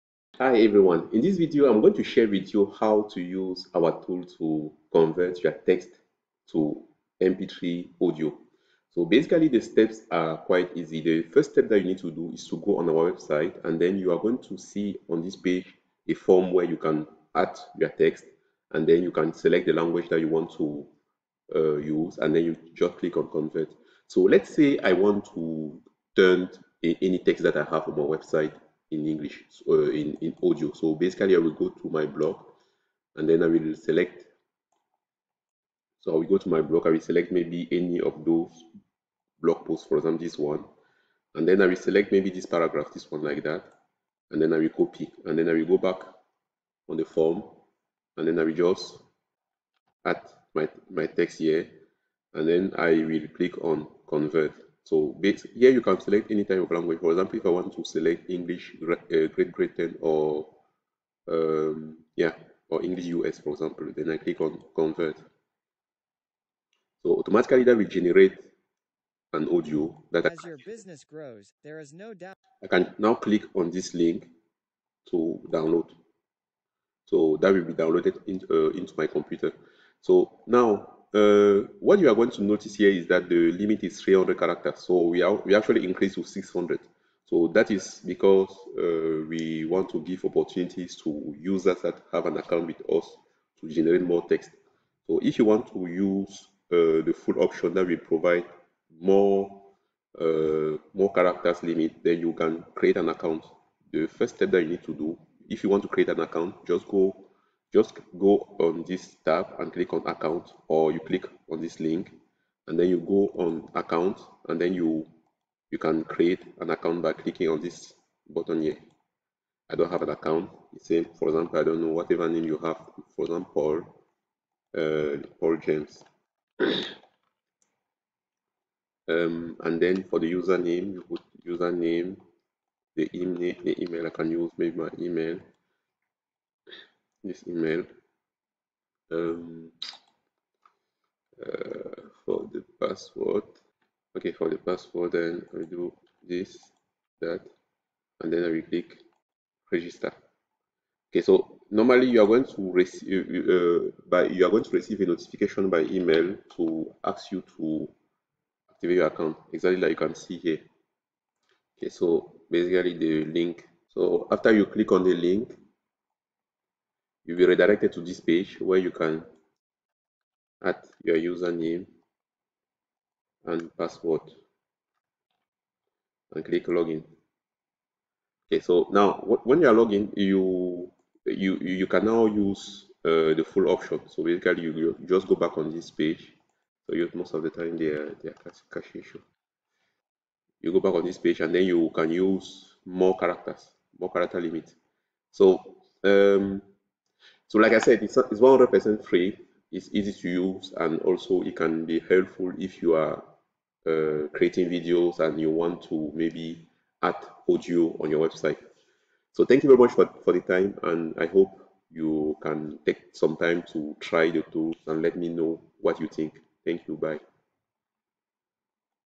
Convert your texts to MP3 audio on our website for free.
2⃣ Paste your text into the form
3⃣ Click on 'convert.'
4⃣ Download your audio file

Without an account, you can convert up to 300 characters of text. Want to convert more? Click on 'Increase the Limit' to log in or create an account for full access to the tool. 🚀

Explore the future of reading with us – your texts are ready to speak! 📚🗣️